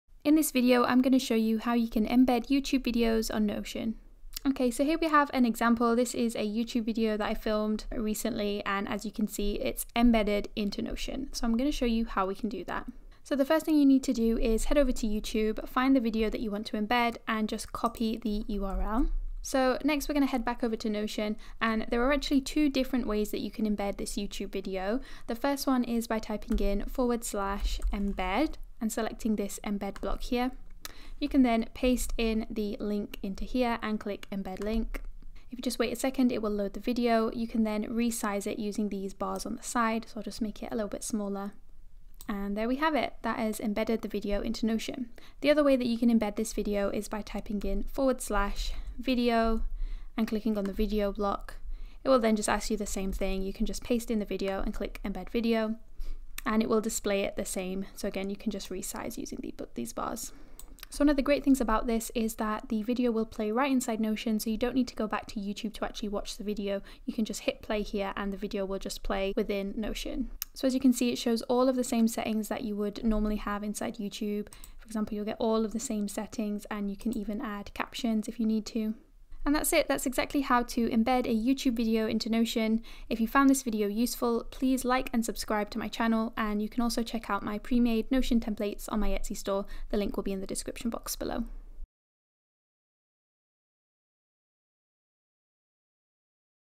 How to Embed a YouTube Video on Notion (Quick Tutorial)
In this video, I show you how you can embed YouTube videos into Notion using the Embed block and the Video block.